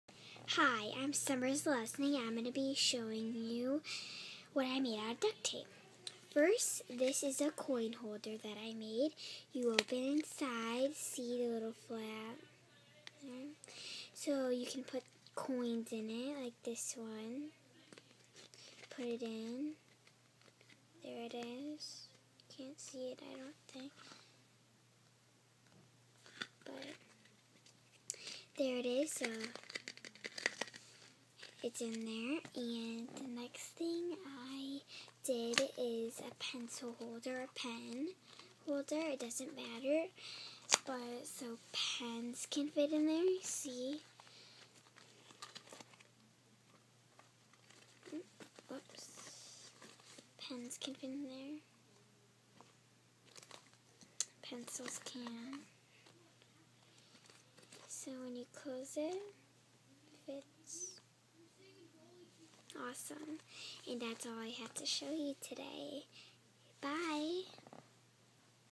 Summer
What Summer has made out of duct tape. 2012